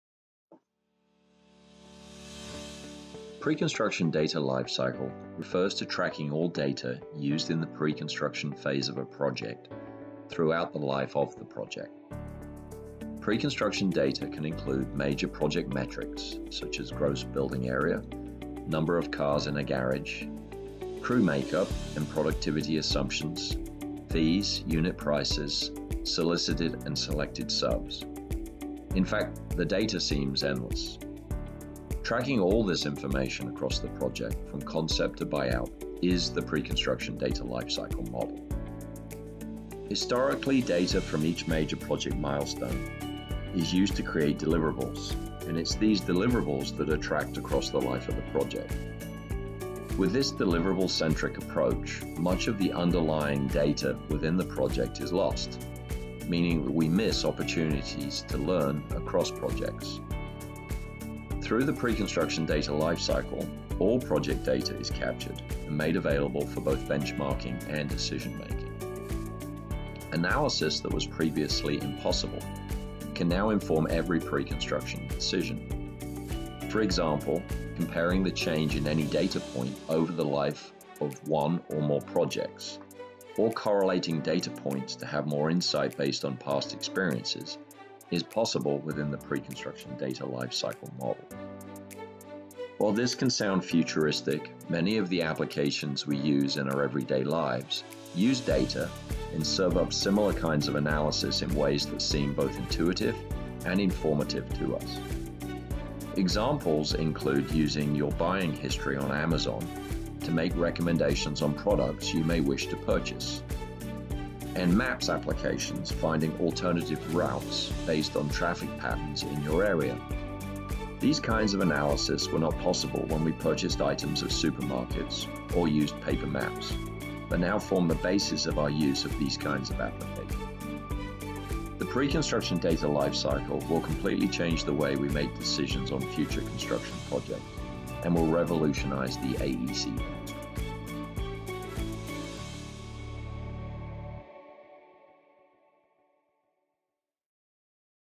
Preconstruction Data Lifecycle
Beck Technology president, Stewart Carroll, defines the preconstruction data lifecycle and how it revolutionizes the construction industry.

To form a complete picture of any project and make better business decisions, an estimator must be able to have all the correct data readily accessible from the beginning of a project to its end. The preconstruction data lifecycle refers to tracking all the data used in the preconstruction phase of a project throughout the life of the project from the conceptual estimating phase to completion. When project data is captured, estimators can use it for benchmarking and better-informed decisions making.

Construction estimating software like Beck Technology’s revolutionary DESTINI Estimator and DESTINI Bid Day automate the capturing, storing, and recalling of all project data allowing preconstruction teams to analyze this data and form solid business decisions and complete the preconstruction data lifecycle.

The concept of preconstruction data lifecycle tools is to track major milestones throughout the project, capture and store that data, and make it easily and quickly accessible for estimators. Just a few examples of how helpful it is to utilize these tools is the ability to look at variances, identify why things change, and compare your conceptual estimate with actuals.

Using innovative preconstruction technologies like estimating software to complete the preconstruction data lifecycle allows us to track key data points and see how data affects every stage of a project. This software means you can create estimate templates based on historical cost data instead of starting from scratch with each estimate.

Preconstruction data lifecycle tools such as DESTINI Estimator allow your preconstruction team to predict costs with greater accuracy and help build trust with owners leading to more project wins and a stellar reputation for staying on budget and delivering on time.